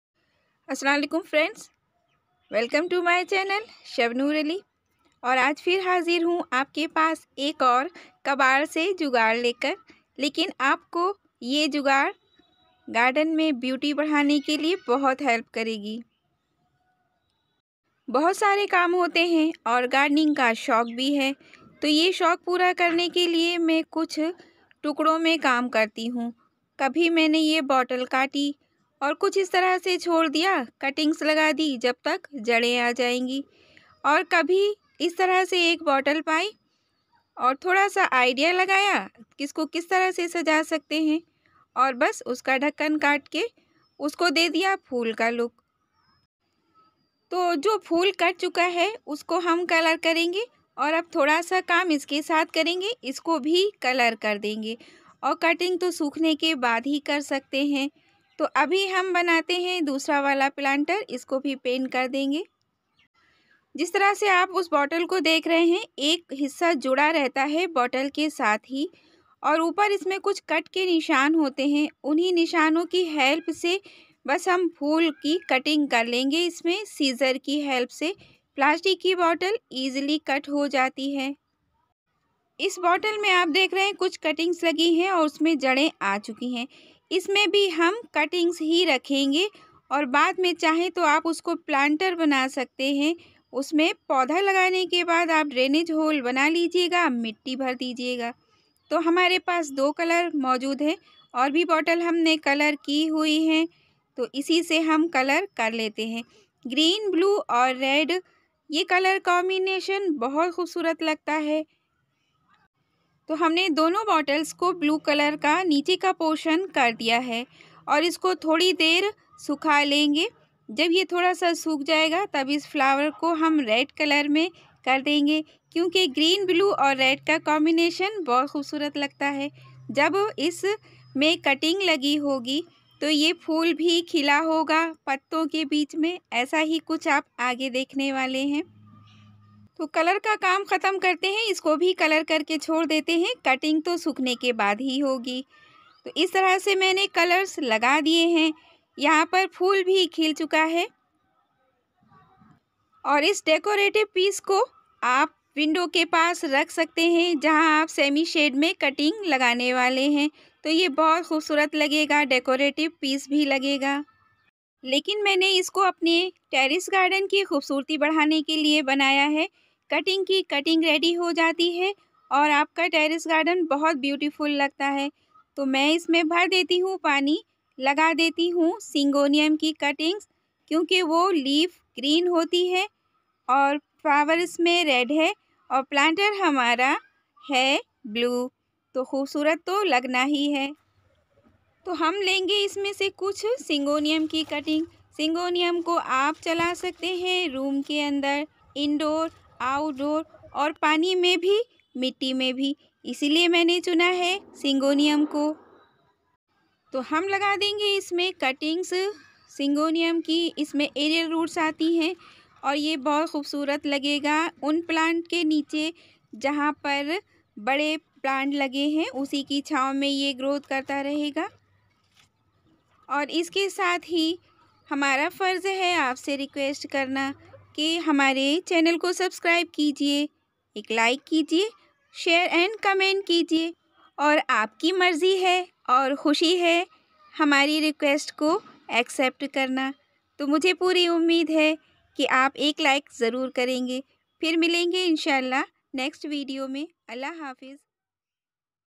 grow any cutting in a good looking waste meterial planter /DIY plastic Planter
Very beautiful colourful waste plastic bottles planters making for growing any type of cutting so easy making a room decorative plant holder Old cold drink bottle planter DIY plastic bottles planters